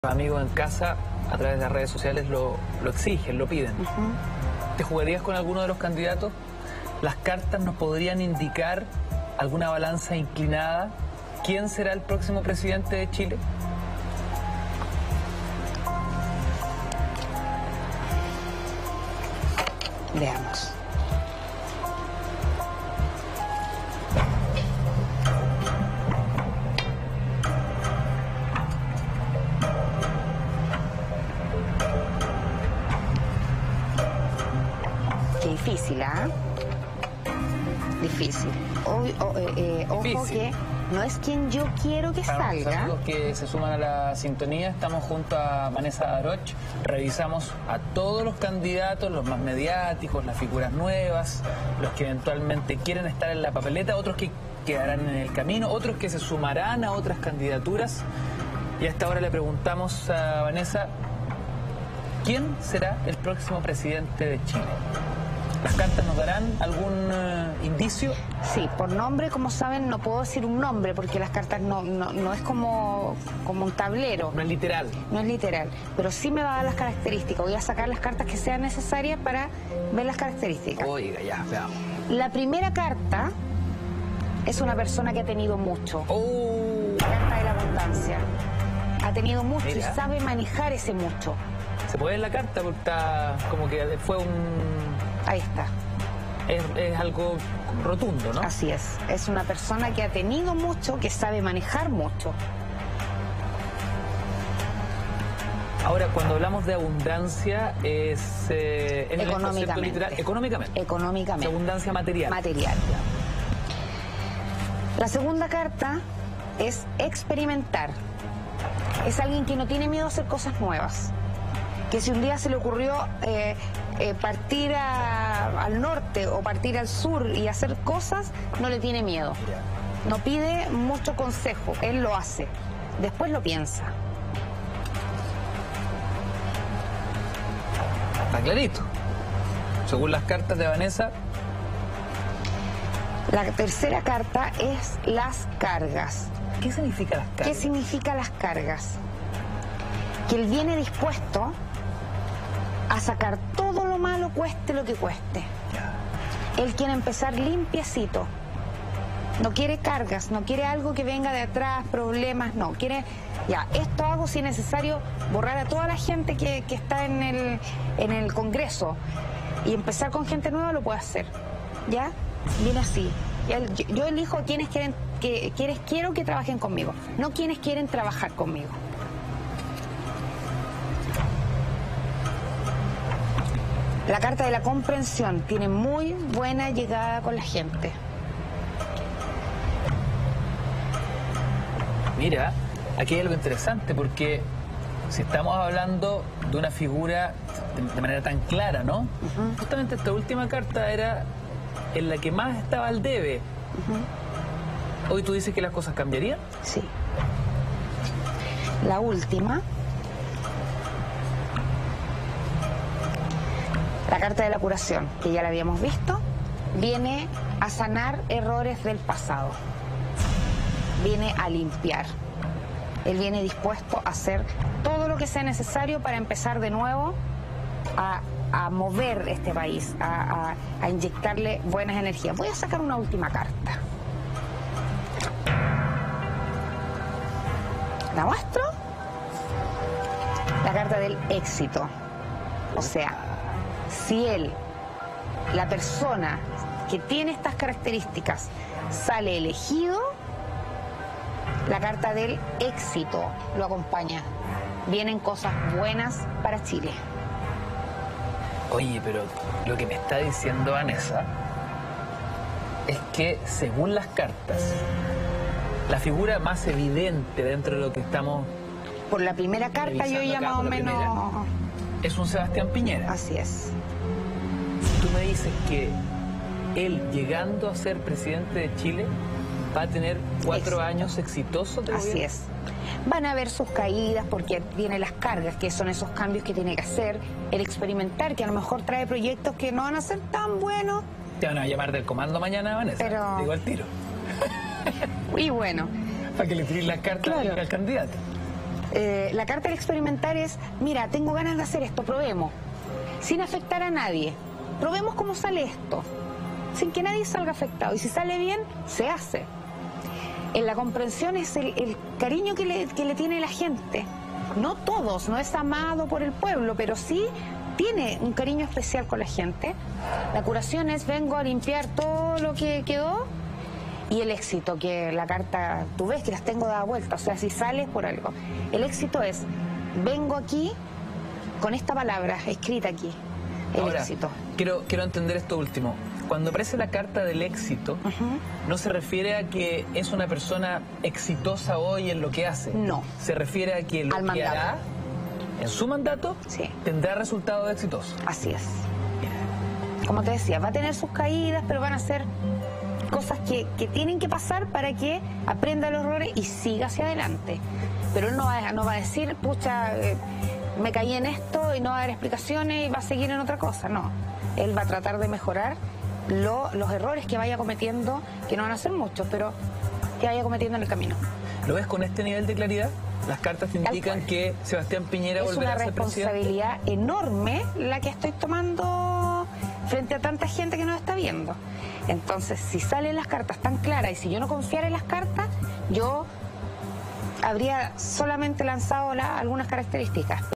Este es el nombre del próximo Presidente de Chile según Vanessa Daroch
La medium sacó las cartas del Tarot, en donde las primeras evidencias indican que Sebastián Piñera podría ser el próximo mandatario y sería muy exitoso.